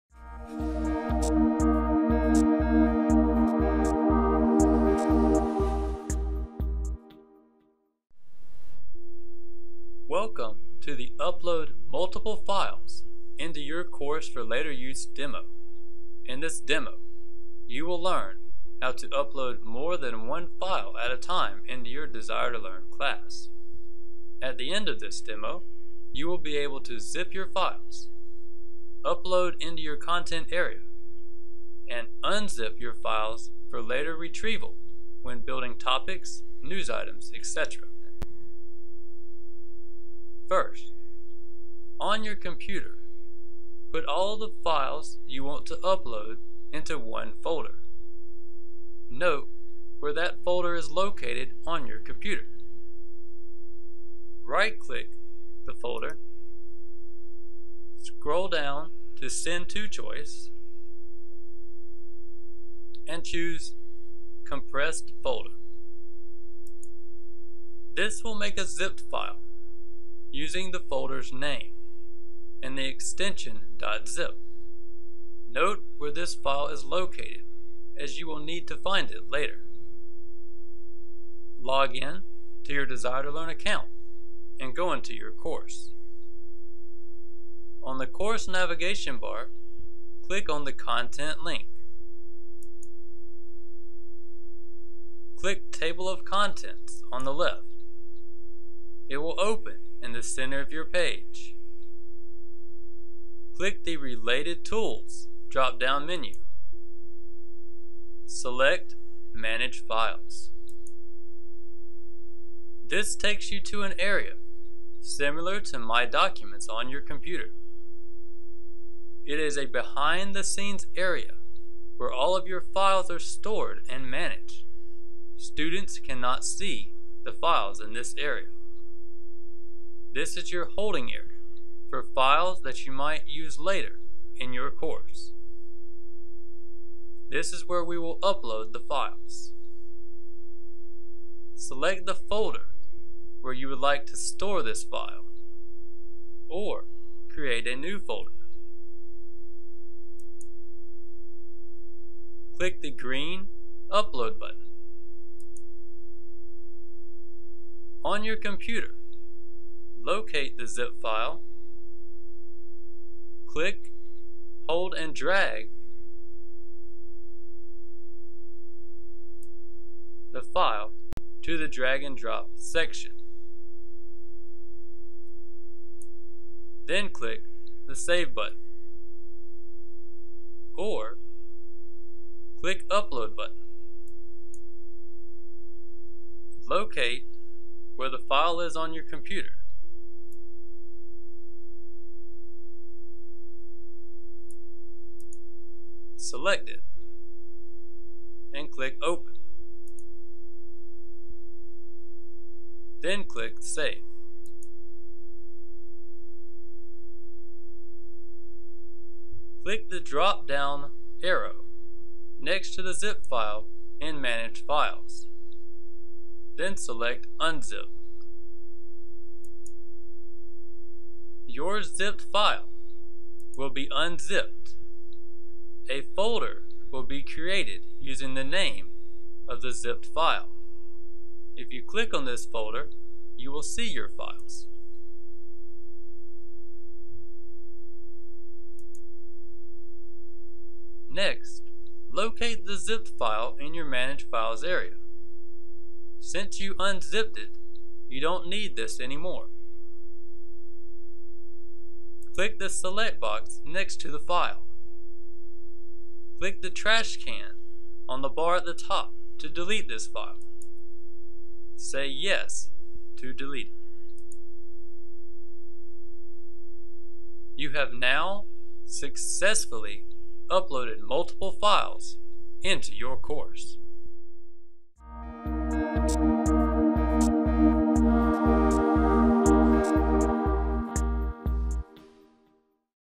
D2L v10.3 Upload Multiple Files into Your Course for Later Use Demo
Welcome to the 'Upload Multiple Files into Your Course for Later Use' Demo.
In this demo, you will learn how to upload more than one file at a time into your D2L class.
At the end of this demo, you will be able to zip your files, upload into your content area, and unzip your files for later retrieval when building topics, news items, etc.

First, on your computer, put all of the files you want to upload into one folder.  Note where that folder is located on your computer.

Right-click that folder, scroll down to the 'Send to' choice, and then choose 'compressed folder'.  This will make a zipped file, using the folder's name, and the extension .zip.  Note where this file is located, as you will need to find it later.

Login to your D2L account and go into your course.
On the course navigation bar: click on the Content link.
Click on Table of Contents on the left.  It will open up in the center of your page.  Click the Related Tools dropdown menu.
Select Manage Files.
This takes you to an area similar to 'My Documents' on your computer.  It is a 'behind the scenes area' where all of your files are stored and managed.  Students can't see files in this area.  This is your 'holding area' for files that you might later use in your course.

This is where we will upload the files.
Select the folder where you would like to store this file, or create a new folder.
Click the Green Upload Button.
On your computer, locate the zipped file you wish to upload and Click, hold and drag the file folder to the drag and drop section.
Then Click Save.
-Or-
Locate the file you wish to upload and Control Select the file folder and click open.
Then Click Save.
Click the down arrow next to Zipped File in your Manage Files. Then select to Unzip.  Your zipped file will be unzipped.  A folder will be created, using the name of the zipped file.  If you click on this folder, you will find your files.

Next, locate the zipped file in your Manage Files area. Since you unzipped it, you don't need this anymore.  Click on the select box next to the file.  Click the trash can on the bar at the top to delete this file.  Click Yes to delete it.
You have now successfully uploaded multiple files into your course.

If you later want to add these files to your course, watch the "Creating a New Topic From a Course File Demo"